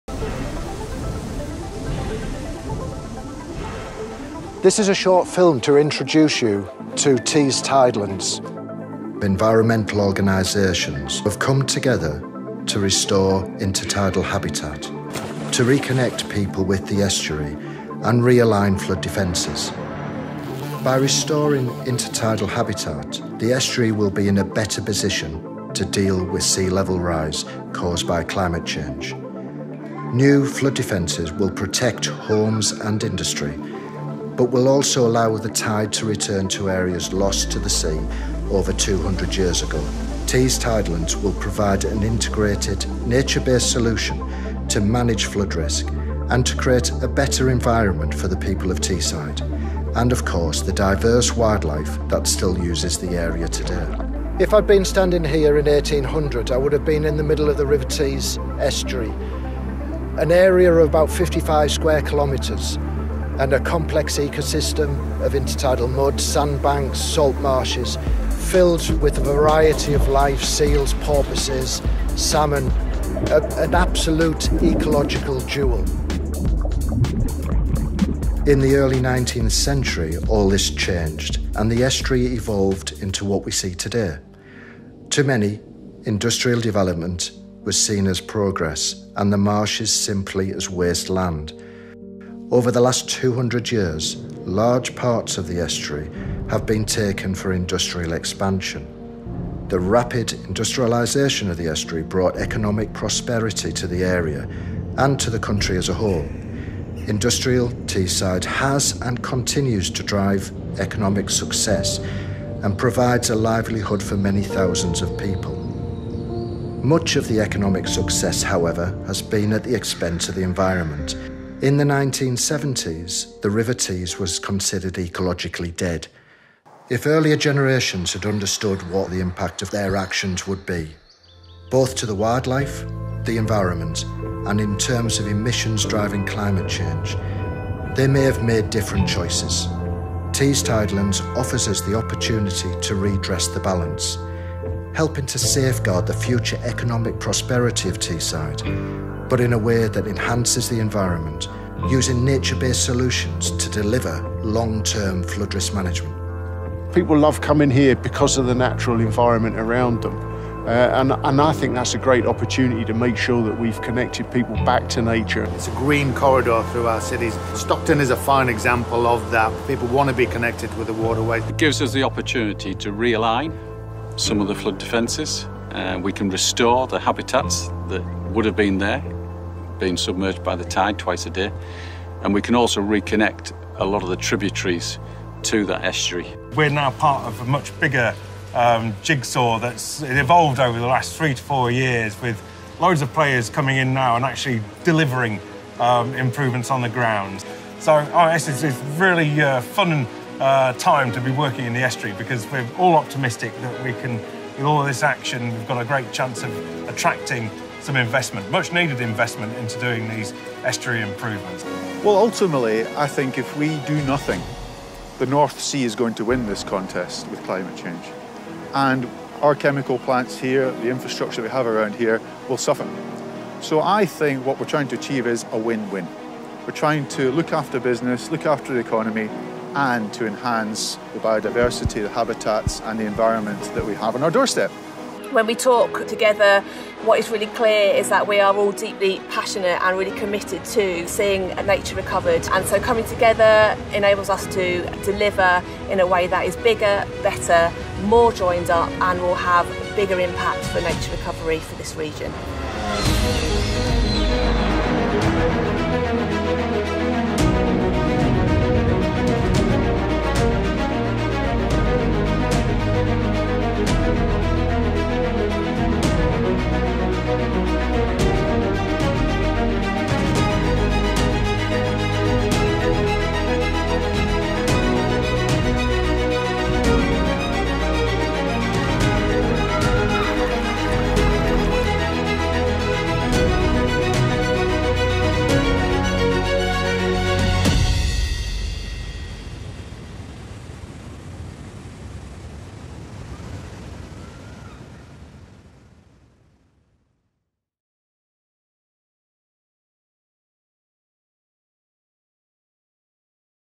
Tees Tidelands - An introduction
A short introduction to the Tees Tidelands Programme, a series of innovative projects which will reduce flood risk, restore valuable habitat and boost biodiversity in Teesside.
The collaborative £30m project will help the River Tees Estuary adapt to climate change and reconnect the river’s tributaries.